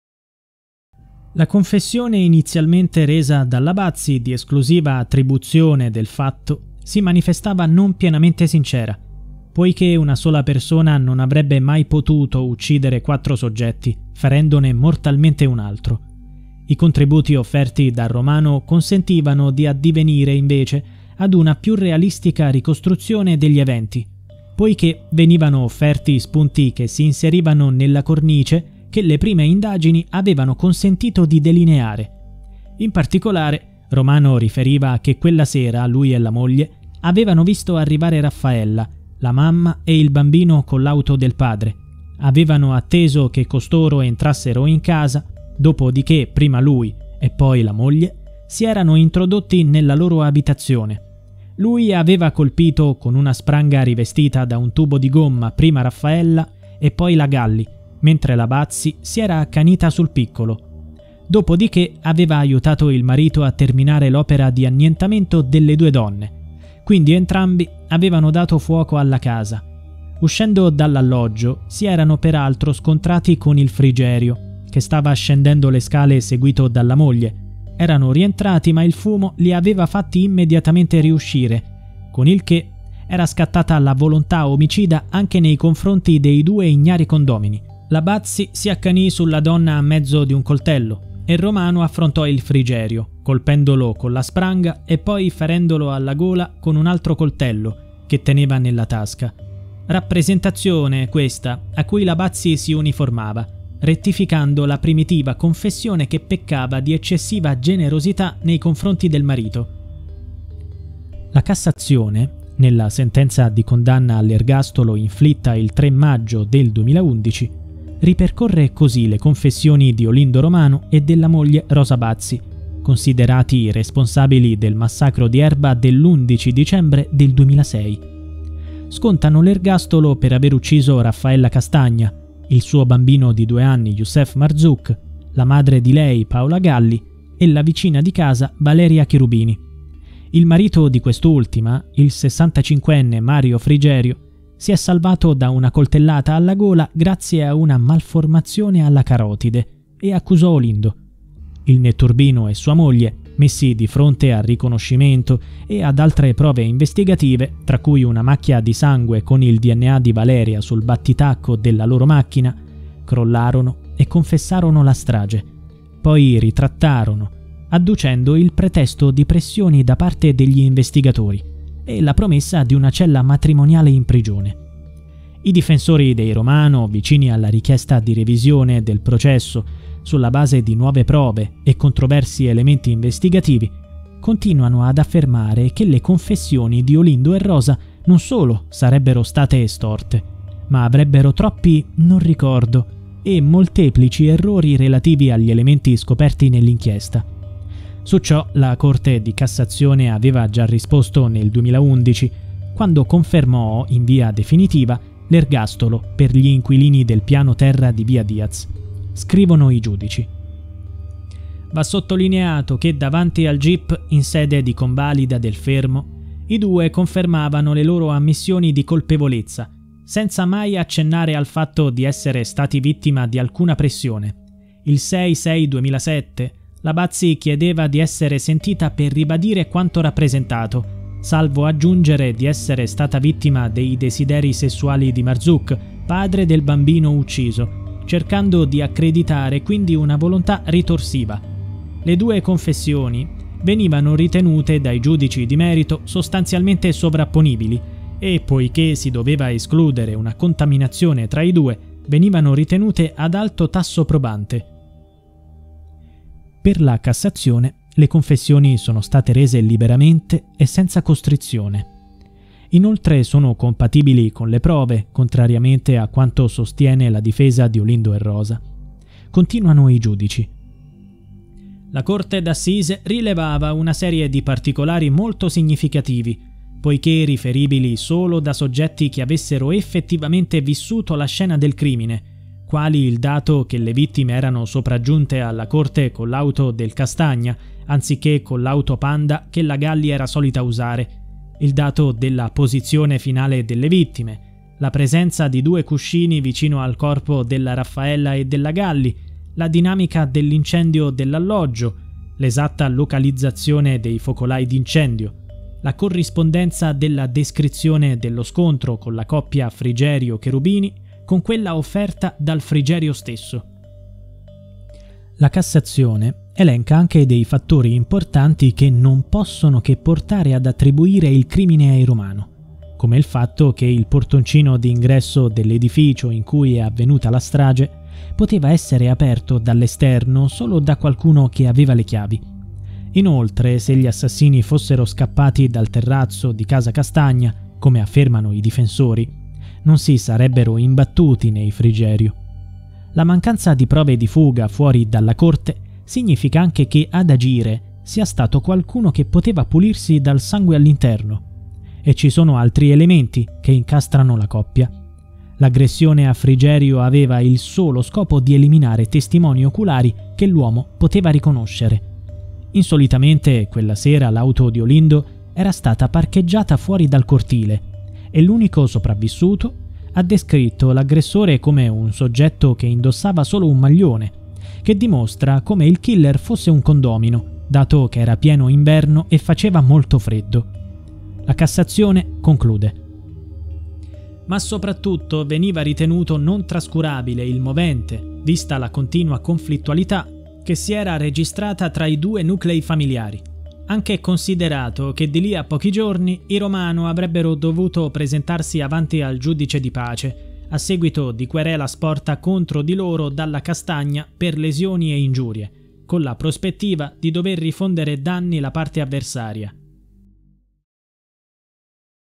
"Incompatibilità con le prove" - Il delitto di Erba
La difesa dei coniugi Romano punta alla revisione del processo